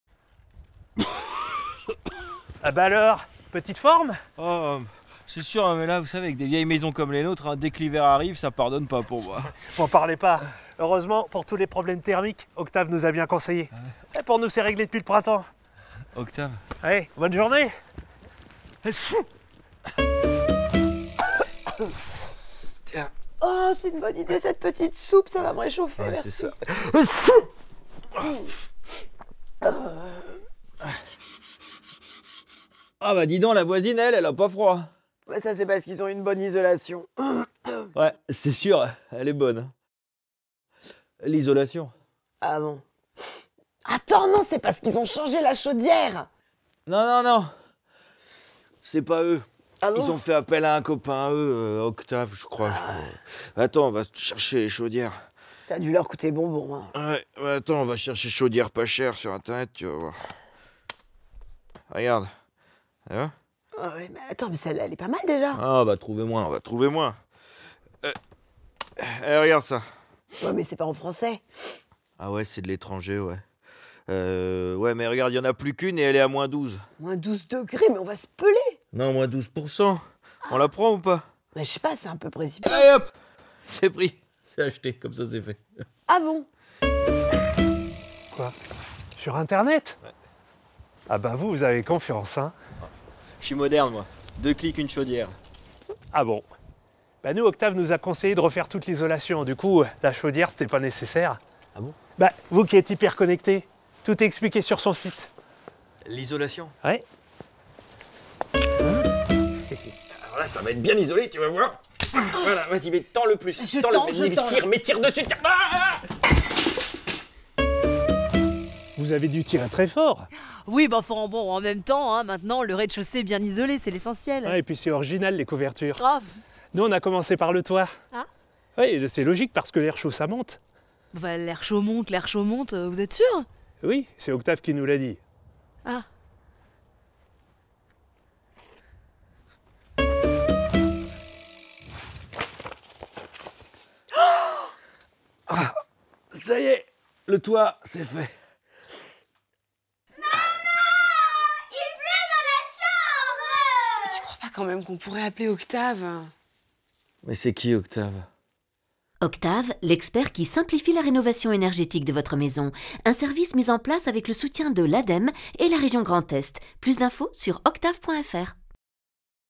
Et si on se réchauffait avec une soupe ? (épisode 2 avec sous-titres) | Oktave
Deuxième épisode de la web-série du dispositif Oktave, pour mieux cerner les enjeux d'une bonne rénovation énergétique globale de sa maison.